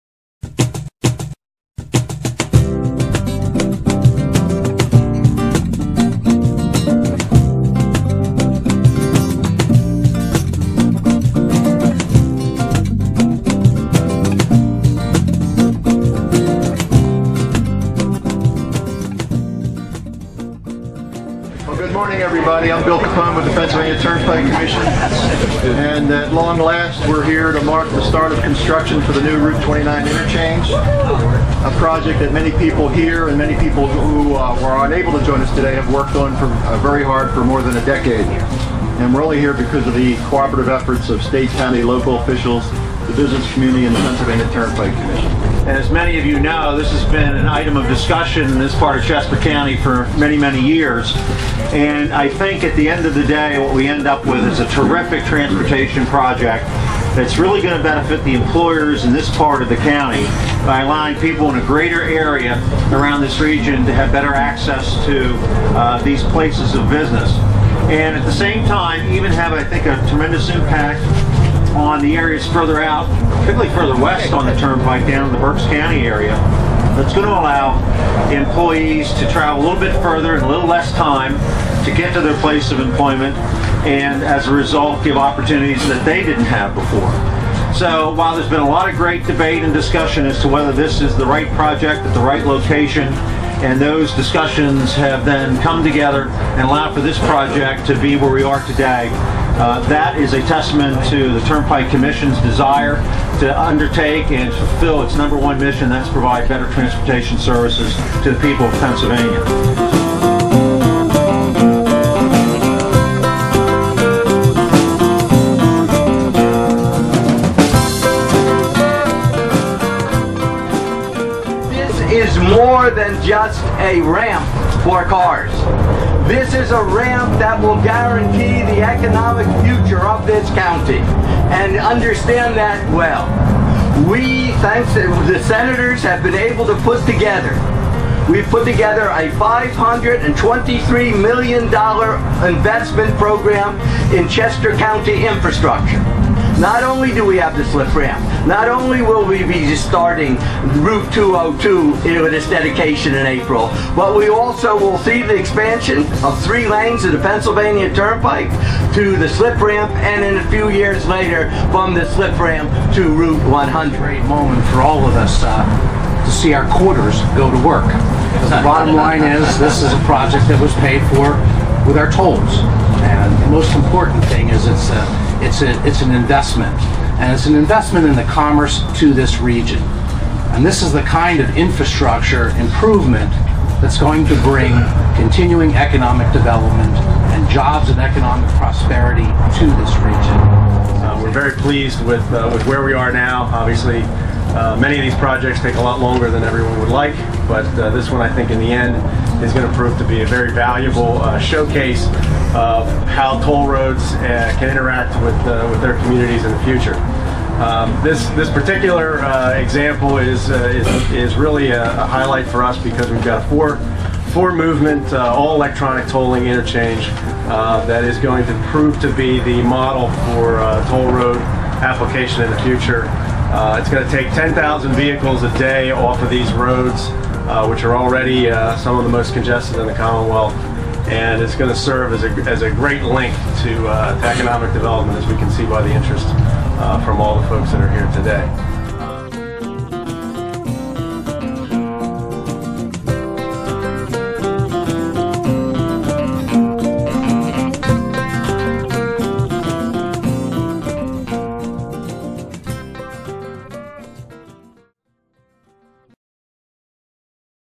Route 29 Slip Ramp Groundbreaking Ceremony
Groundbreaking Ceremony for the Route 29 Slip Ramp held March 2011 in Great Valley.